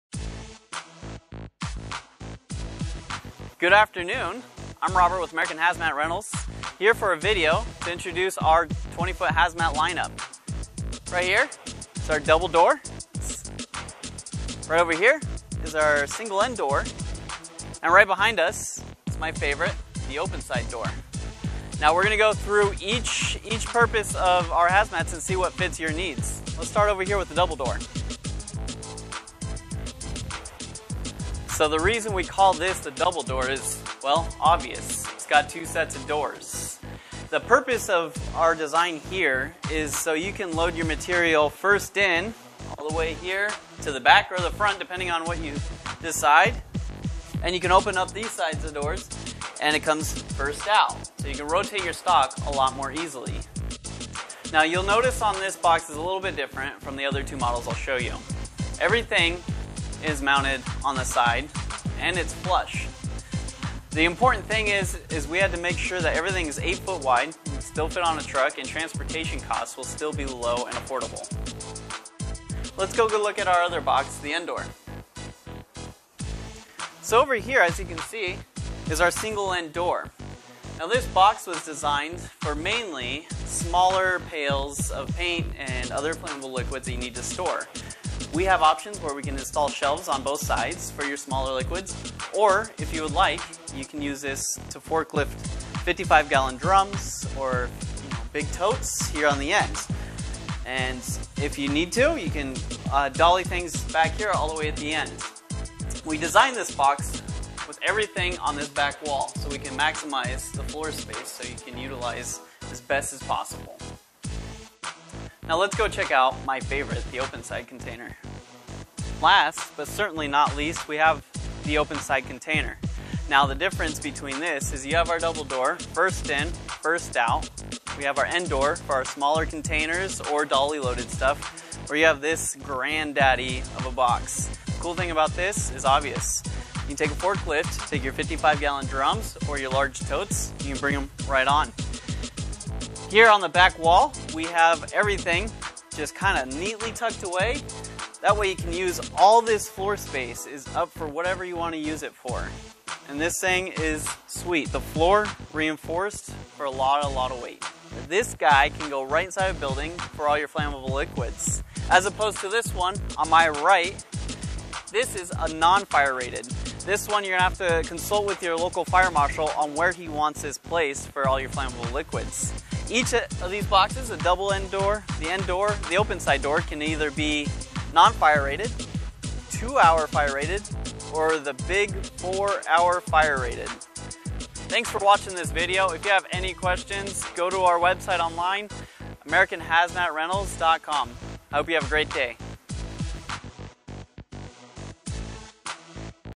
3 different Hazmat styles
In this video we explain the difference between our single and door, double end door, and open side 20 ft portable Hazmat containers available for rent within the United States.